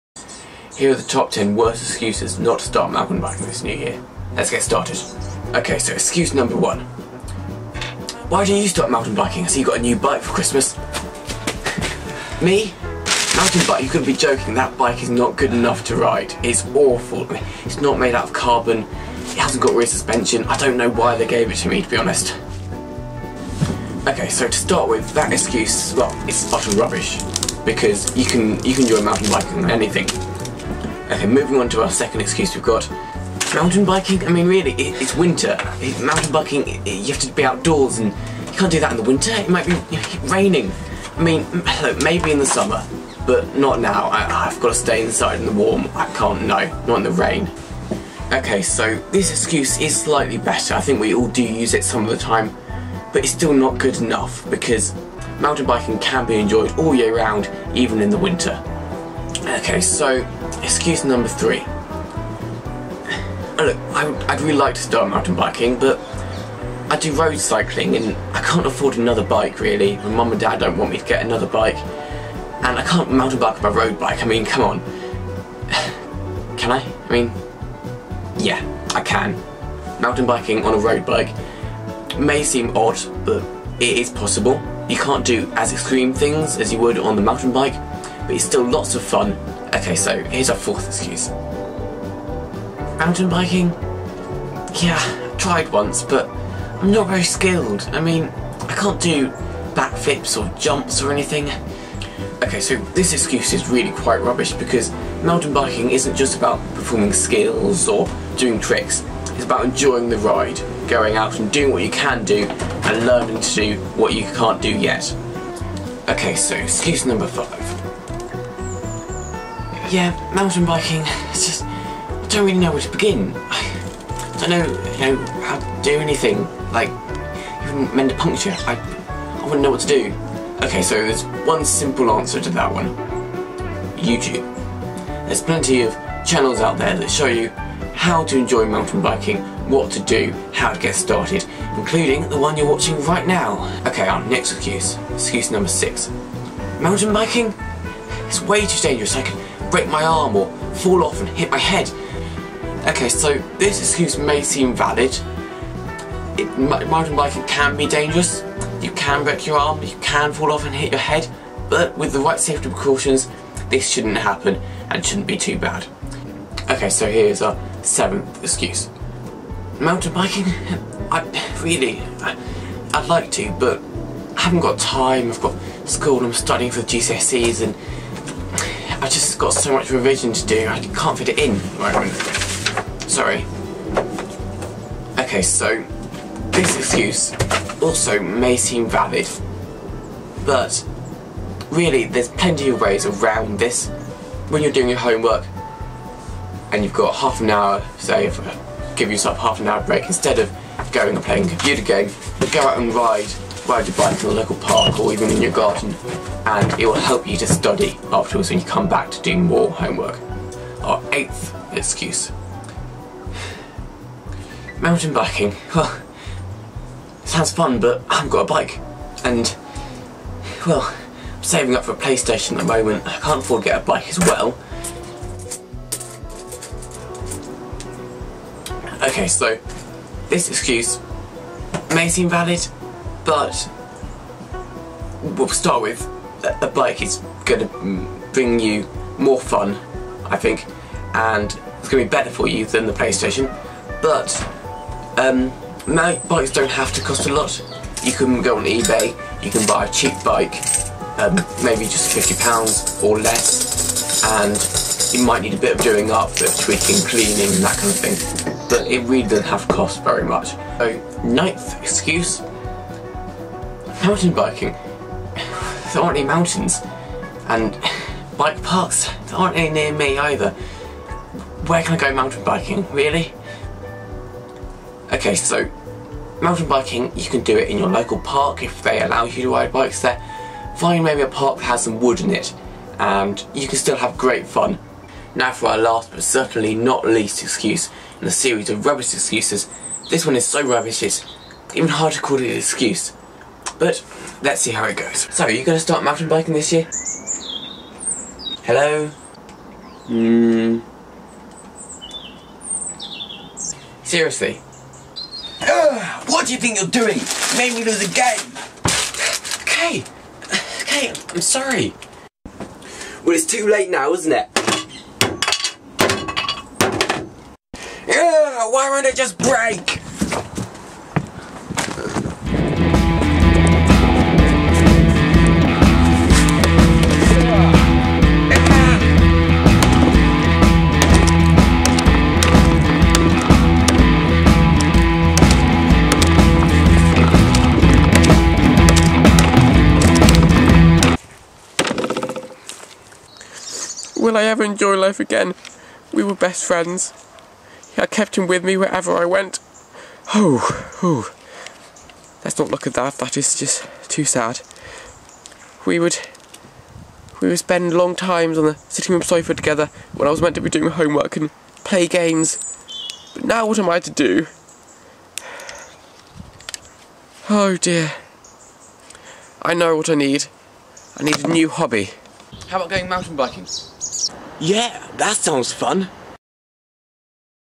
Top ten worst excuses not to start mountain biking
The new year is a time to start new hobbies . why not start mountain biking?